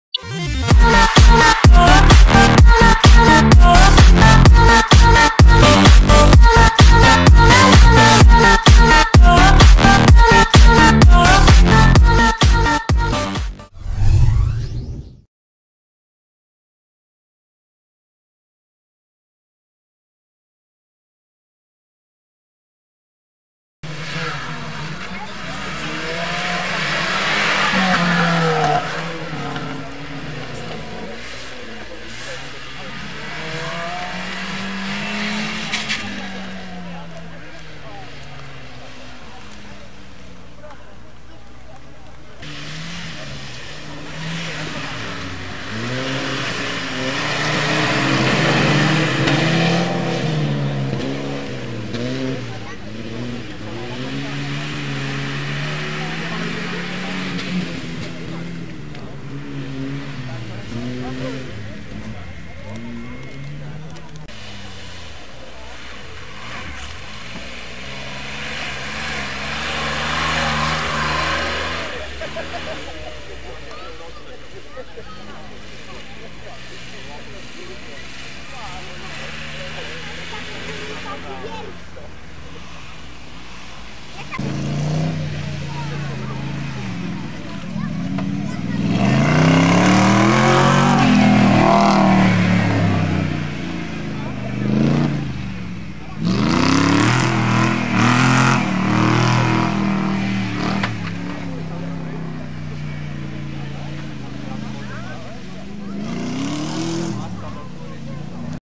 Rajd Korczyna 4x4 2014
ZLOT SAMOCHODÓW TERENOWYCH 4x4 CZARNORZEKI - KORCZYNA - PUCHAR PODKARPACIA - OFF ROAD 2014 / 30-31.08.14r.